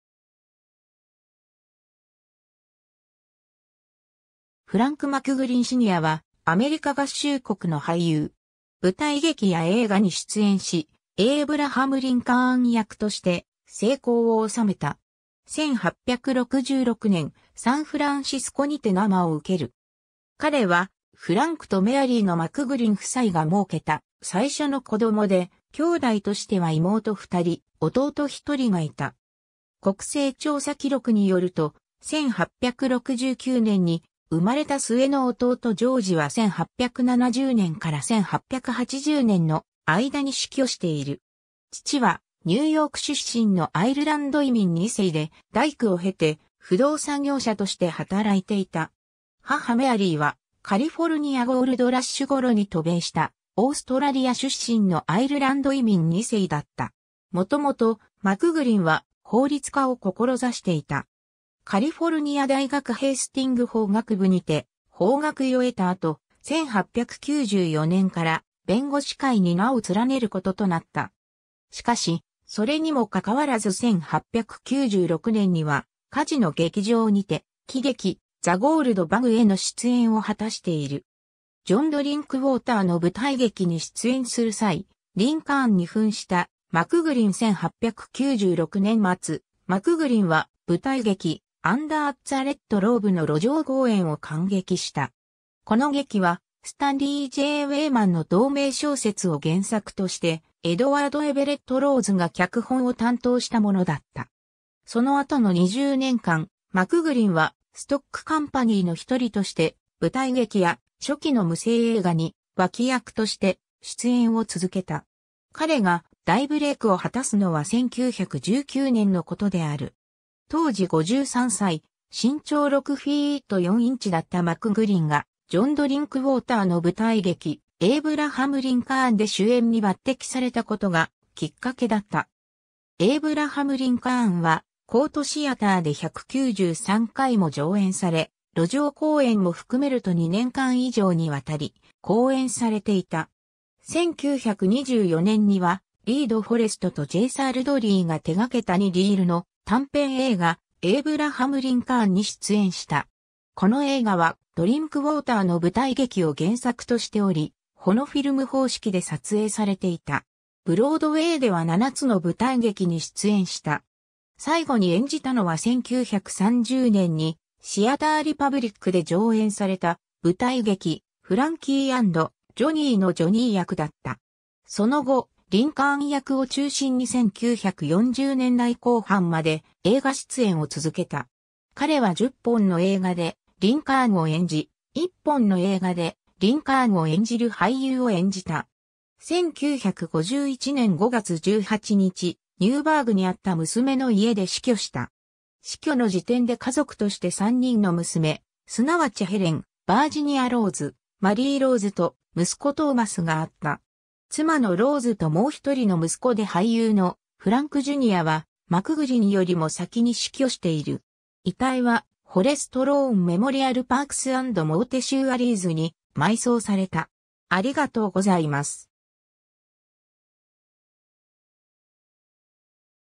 フランク・マクグリン・シニア
エイブラハム・リンカーン
フランク・マクグリン・シニア（Frank McGlynn Sr., 1866年10月26日 - 1951年5月18日）は、アメリカ合衆国の俳優。
舞台劇や映画に出演し、エイブラハム・リンカーン役として成功を収めた。
 1866年、サンフランシスコにて生を受ける。
彼はフランクとメアリーのマクグリン夫妻がもうけた最初の子供で、兄弟としては妹2人、弟1人がいた。
国勢調査記録によると、1869年に生まれた末の弟ジョージは1870年から1880年の間に死去している。
父はニューヨーク出身のアイルランド移民二世で、大工を経て不動産業者として働いていた。
母メアリーはカリフォルニア・ゴールドラッシュ頃に渡米したオーストラリア出身のアイルランド移民二世だった。
 元々、マクグリンは法律家を志していた。
カリフォルニア大学ヘイスティング法学部にて法学位を得た後、1894年から弁護士会（Bar）に名を連ねることとなった。
しかし、それにもかかわらず1896年にはカジノ劇場にて喜劇『The Gold Bug』（脚本：グレン・マクドナウ、音楽：ヴィクター・ハーバート）への出演を果たしている。
 ジョン・ドリンクウォーターの舞台劇に出演する際、リンカーンに扮したマクグリン（1920年頃） 1896年末、マクグリンは舞台劇『Under the Red Robe』の路上公演を観劇した。
この劇はスタンリー・J・ウェイマンの同名小説を原作としてエドワード・エヴェレット・ローズが脚本を担当したものだった。
その後の20年間、マクグリンはストック・カンパニー（Stock company, 劇場住込み俳優）の1人として舞台劇や初期の無声映画に脇役として出演を続けた。
 彼が大ブレイクを果たすのは1919年のことである。
当時53歳、身長6フィート4インチだったマクグリンが、ジョン・ドリンクウォーターの舞台劇『Abraham Lincoln』で主演に抜擢されたことがきっかけだった。
『Abraham Lincoln』はコート・シアターで193回も上演され、路上公演も含めると2年間以上にわたり公演されていた。
1924年にはリー・ド・フォレストとJ・サール・ドーリーが手がけた2リールの短編映画『Abraham Lincoln』に出演した。
この映画はドリンクウォーターの舞台劇を原作としており、フォノフィルム方式で撮影されていた。
ブロードウェイでは7つの舞台劇に出演した。
最後に演じたのは1930年にシアター・リパブリックで上演された舞台劇『Frankie and Johnnie』のジョニー役だった。
その後、リンカーン役を中心に1940年代後半まで映画出演を続けた。
彼は10本の映画でリンカーンを演じ、1本の映画でリンカーンを演じる俳優を演じた。
 1951年5月18日、ニューバーグにあった娘の家で死去した。
死去の時点で家族として3人の娘、すなわちヘレン（Helen）、バージニア・ローズ（Virginia Rose）、マリー・ローズ（Mary Rose）と、息子トーマス（Thomas）があった。
妻のローズ（Rose）ともう1人の息子で俳優のフランク・ジュニア（Frank Jr.）は、マクグリンよりも先に死去している。
遺体はForest Lawn Memorial-Parks & Mortuariesに埋葬された。